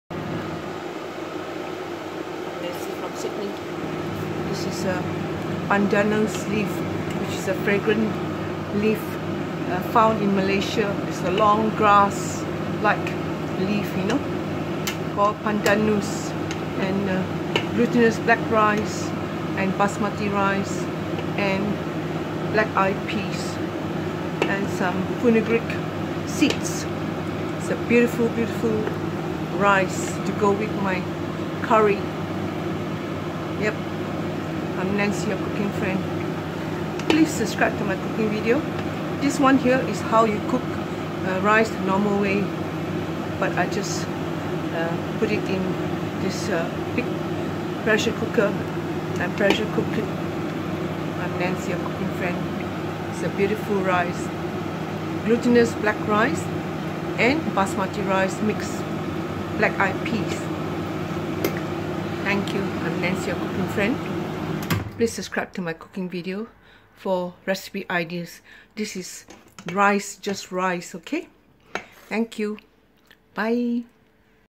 Pressure steamed Basmati brown rice, Glutinous black rice.
1 cup Banasmati rice, 1 cup Glutinous black rice washed, 1 teasp. Fenugreek seeds, enough tap water one inch above washed rice! Pinch of Himalayan salts(optional).